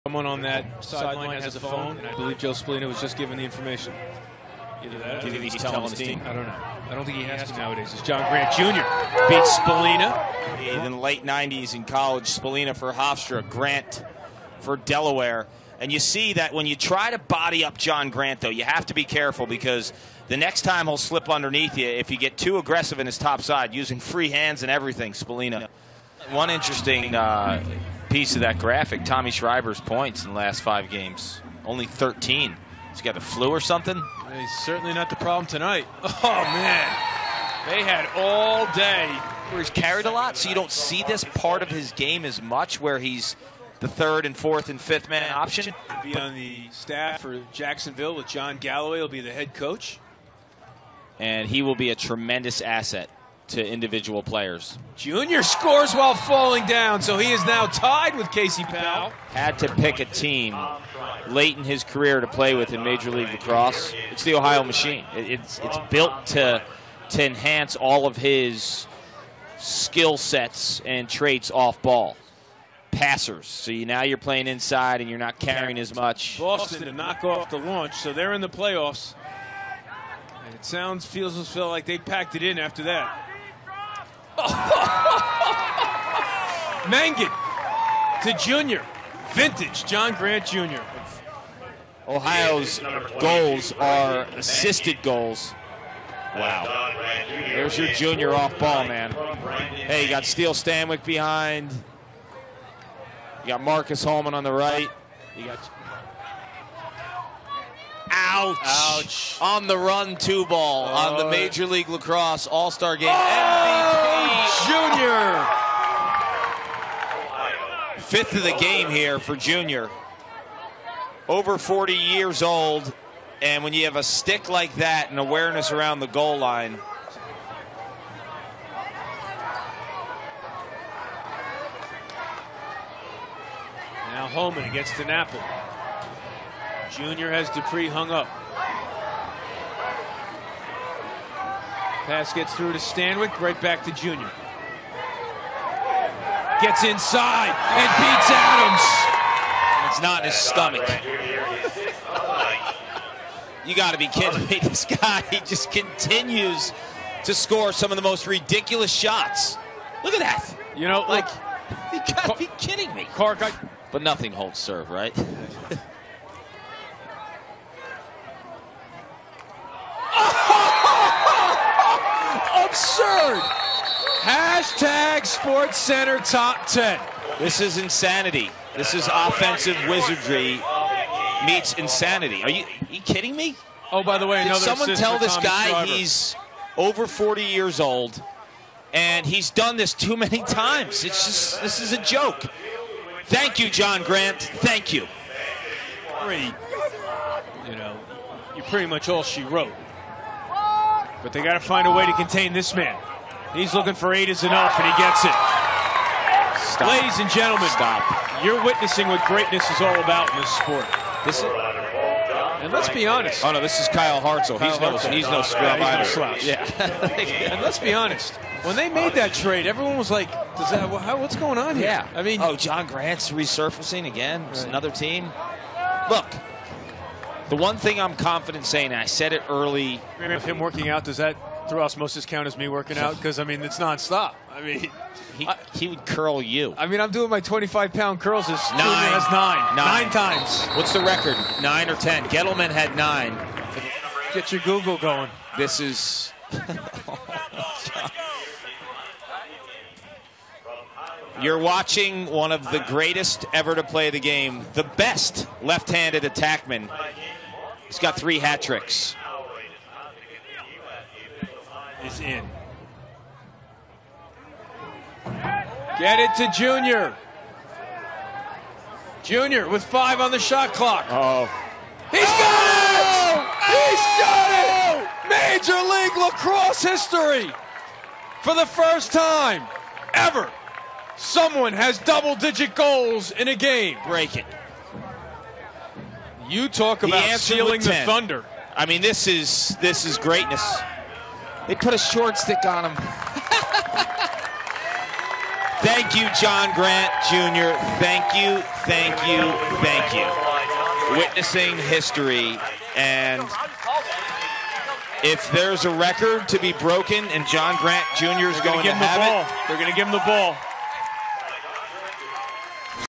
John Grant Jr. scores 10 goals in a game to set the MLL record
Ohio Machine attackman John Grant Jr. scores 10 goals against the NY Lizards to set the Major League Lacrosse record for goals in a single game.